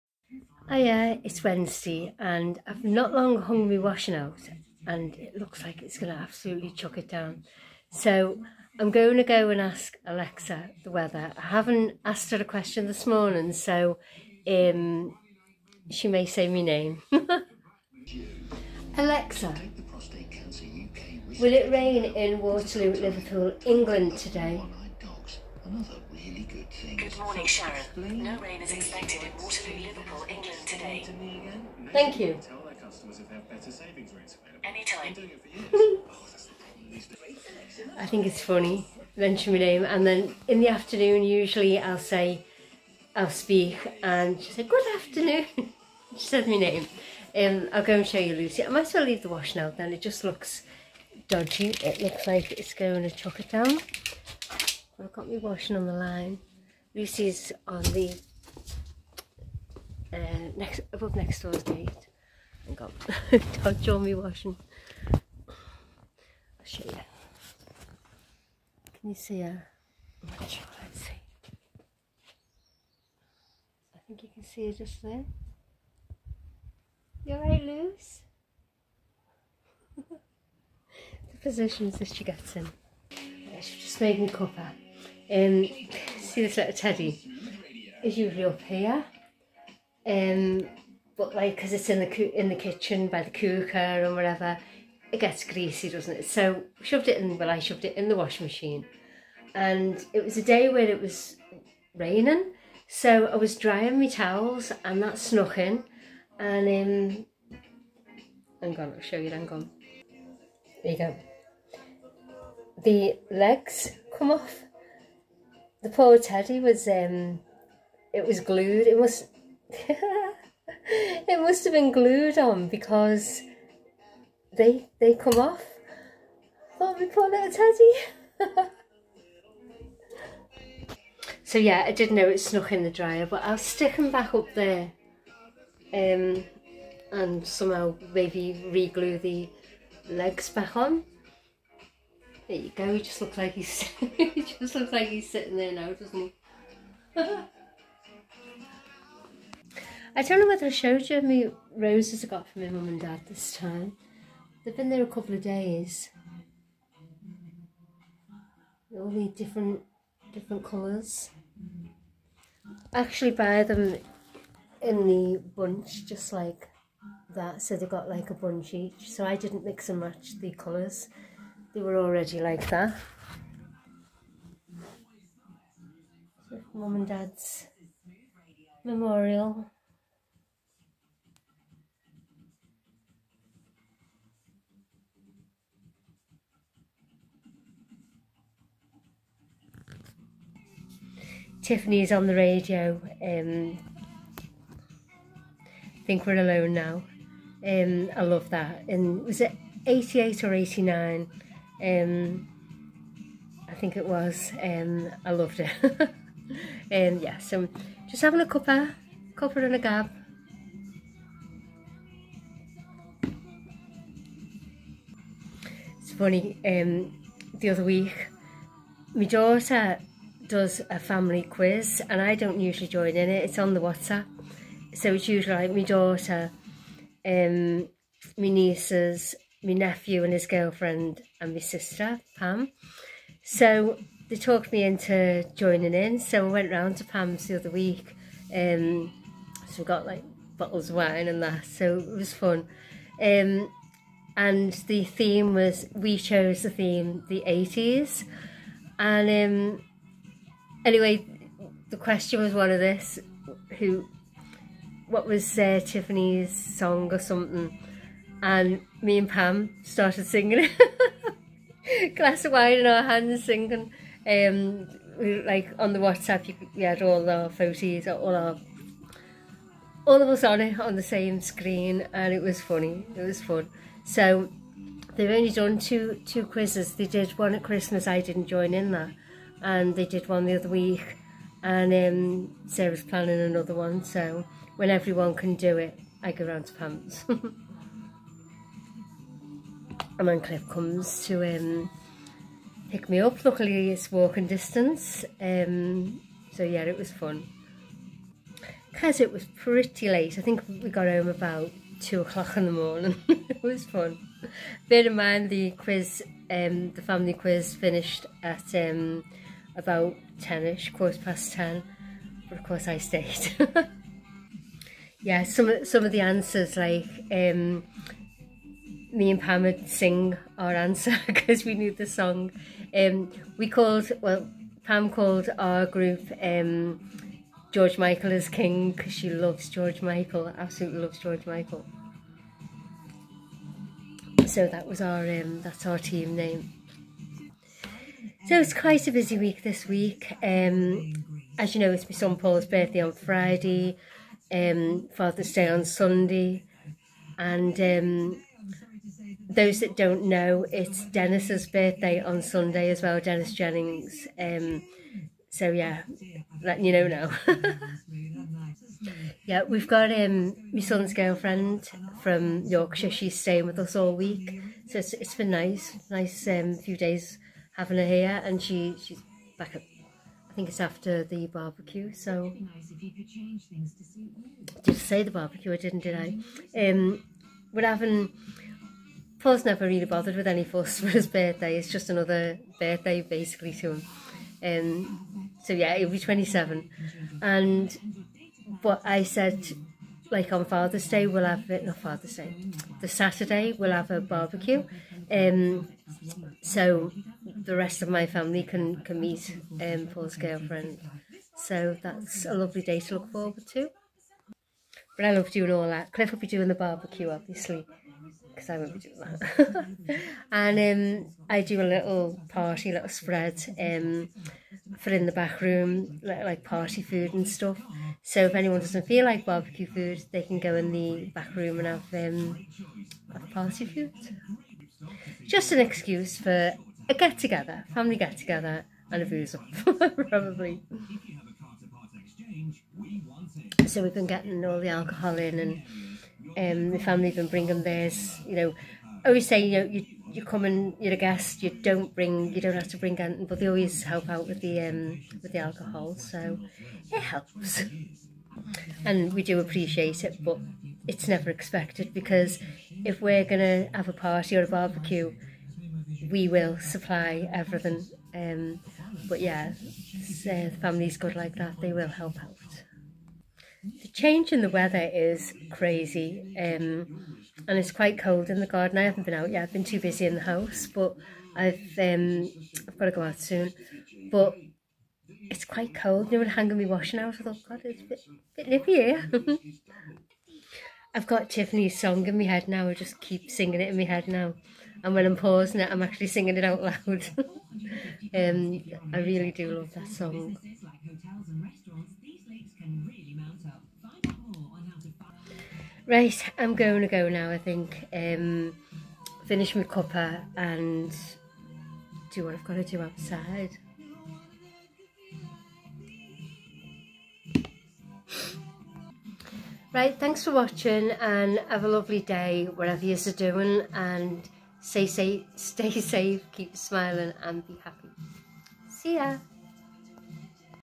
Wednesday Chat 🗣️👄(Featuring Mum & Dad's Memorial 🌹🕯️& a Cameo Appearance from Lucy 😻)😺12/06/24
Alexa says my name,& it won't rain ⛅😅 0:18 😅⛅
This Teddy 🧸 Bear snuck in the dryer & his legs came off.😱😅🧸 1:24 🧸😅😱
Lucy 😻 1:08 😻
Mum & Dad's Memorial 🕯️ 🌹 2:29 🌹🕯️
Cuppa ☕ & a gab 🗣️ 👄 3:10 👄🗣️☕
I talk about my Daughter arranging a "WhatsApp Family Quiz", but I forgot to mention her Fiancé joins in too 😅 3:39  😅